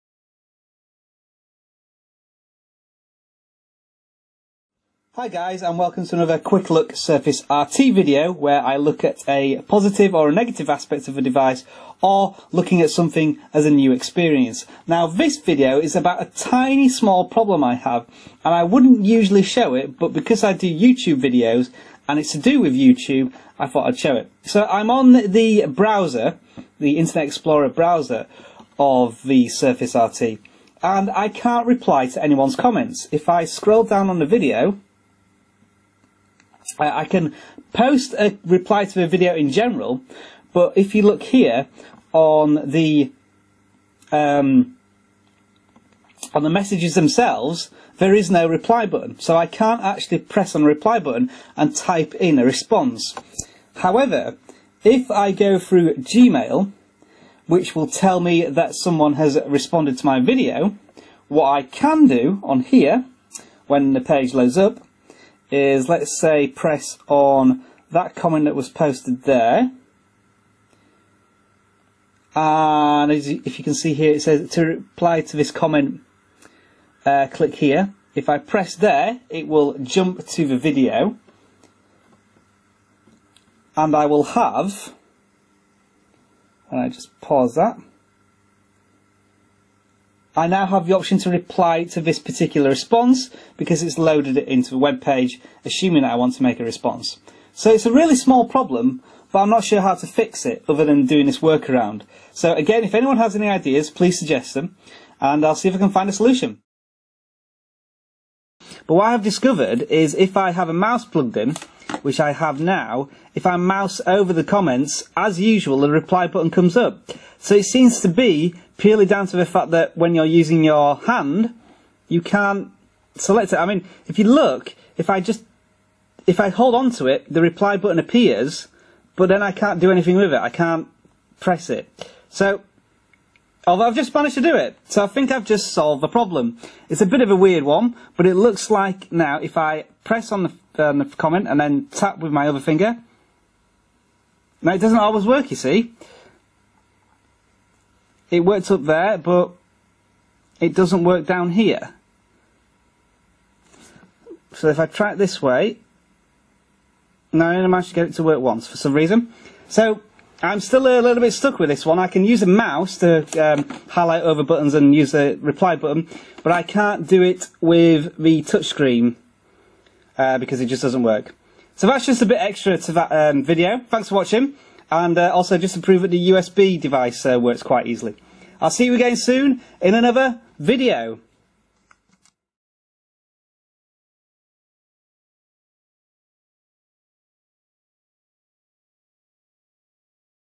Review # 14: Quick Look Youtube Reply Button Microsoft Windows Surface RT
As this is my first experience of the Windows 8 tablet experience and it is, on the whole, a new operating system, I think it's interesting and important to note all the positive and negatives things and how I apply my own learning techniques and logic to work out how this tablet works.

Today is a quick look at an odd problem with the youtube reply button on the browser.